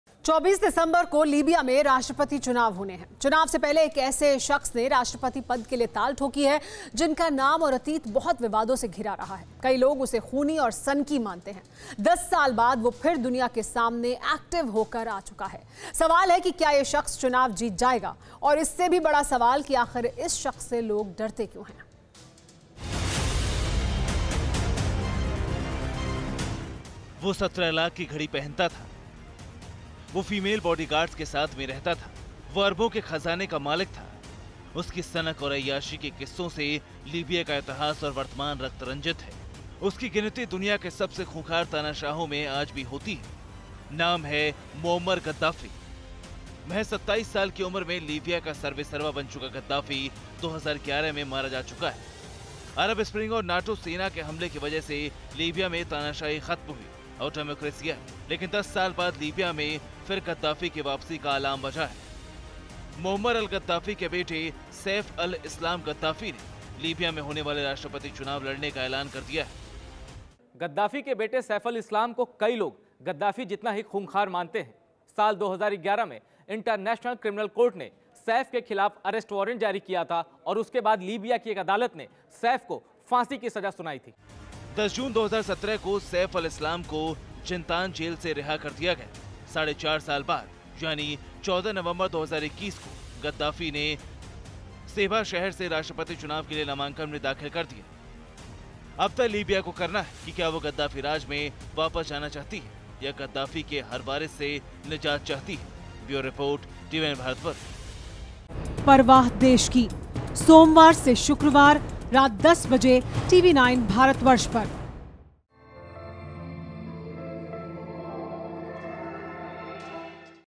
Libyan Dictator Gaddafi का ‘Wanted’ बेटा लड़ेगा President Election, मिल चुकी है मौत की सजा
24 दिसंबर को लीबिया में राष्ट्रपति चुनाव होने हैं. चुनावों से पहले एक ऐसे ऐसे शख्स ने राष्ट्रपति पद के लिए ताल ठोकी है. जिसका नाम और अतीत बहुत विवादों से घिरा है. कई लोग उसे खूनी और सनकी मानते हैं. 10 साल बाद वो फिर दुनिया के सामने एक्टिव होकर आ चुका है.